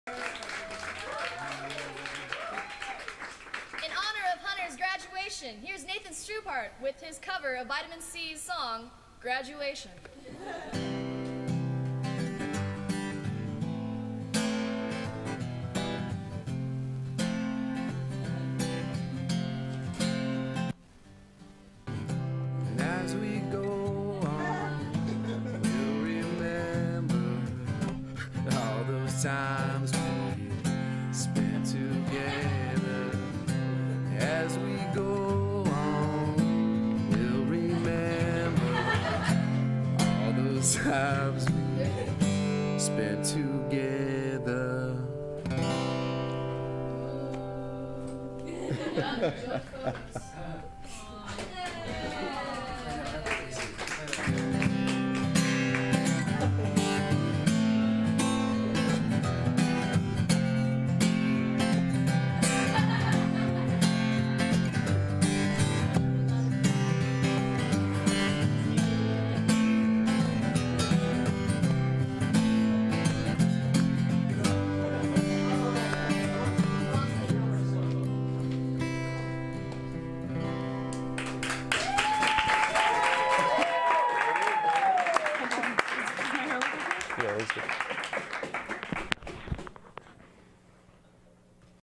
LHLN - Hunter's Graduation Photos - Longhorn LateNight
As we reminisce Hunter's time with us, we look back at all the wonderful moments he captured...for Facebook profile pictures..